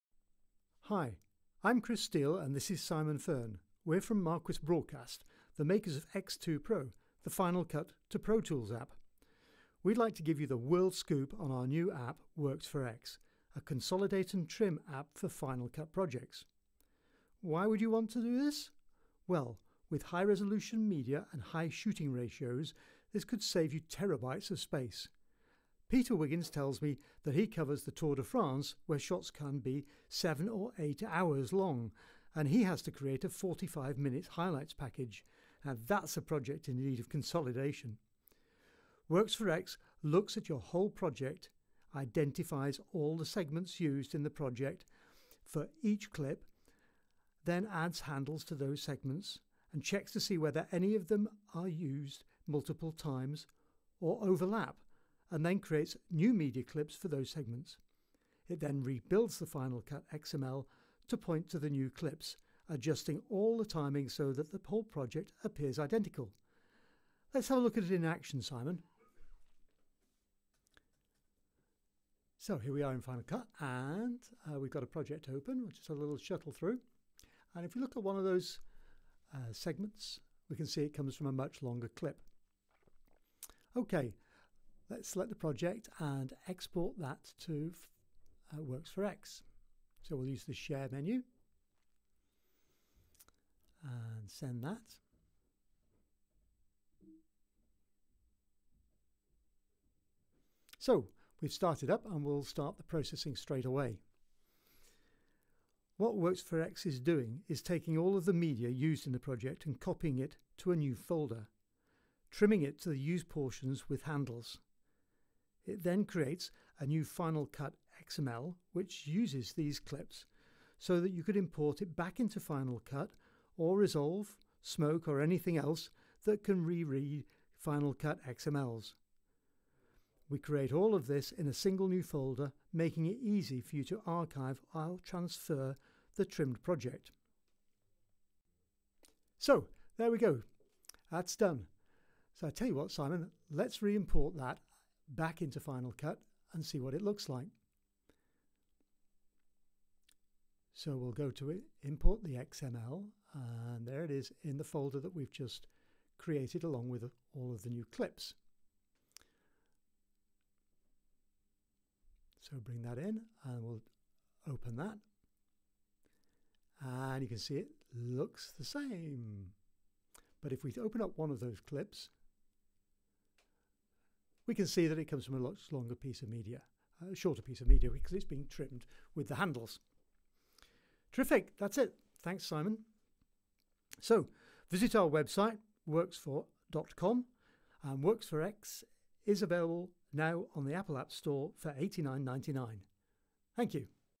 Introduction to Worx4 X
Apple Final Cut Pro X companion that creates a folder of the trimmed media